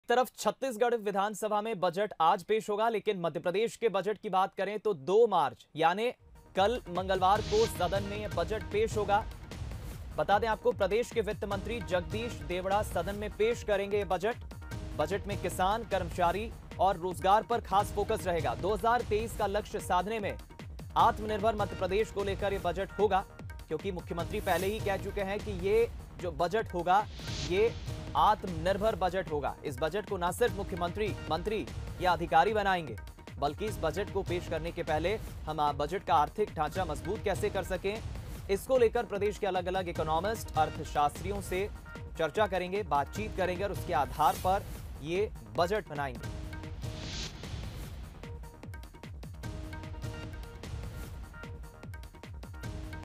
Madhya Pradesh Budget : कल विधानसभा में पेश होगा 2021-22 का आम बजट
भोपाल : कल विधानसभा में पेश होगा 2021-22 का आम बजट
वित्त मंत्री जगदीश देवड़ा सदन में पेश करेंगे बजट
बजट में किसान, कर्मचारी और रोजगार पर रहेगा फोकस
2023 का लक्ष्य साधने के लिए मिशन मोड में चलेंगी योजनाएं